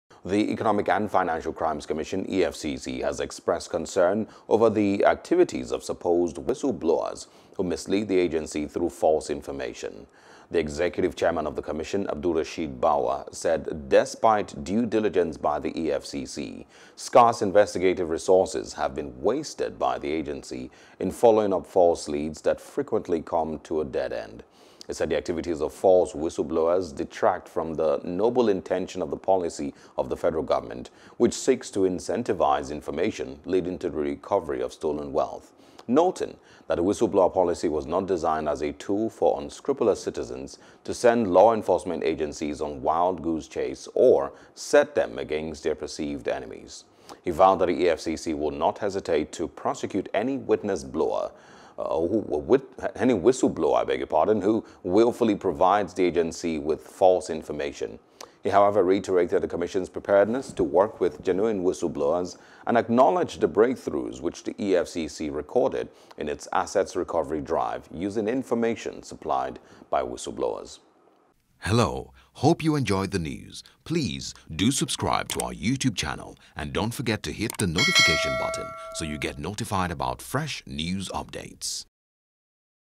EFCC Warns ‘Whistleblowers’ Against Misleading Information | NEWS
The Economic and Financial crimes Commission, EFCC, has expressed concern about the activities of supposed whistleblowers who mislead the agency through false information.